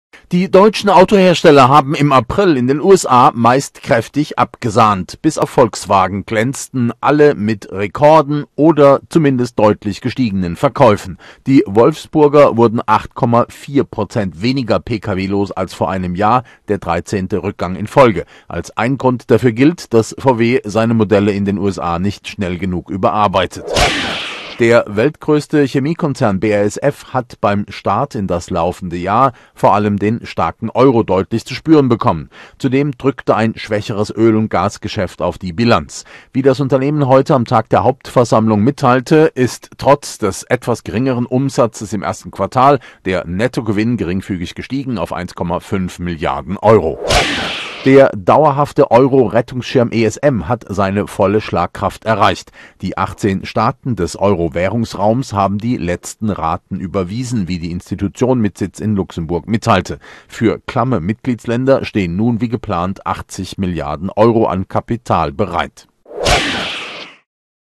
60 Sekunden Wirtschaft am 02.05.2014
Alle Autobauer glänzen in den USA außer Volkswagen - Trotz schwächerem Ölgeschäft: BASF steigert Gewinn - Krisenfest: Euro-Rettungsschirm ESM meldet volle Kassen

                                        60 Sekunden Wirtschaft am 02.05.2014

                                        60 Sekunden Wirtschaft am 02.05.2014

                                        60 Sekunden Wirtschaft am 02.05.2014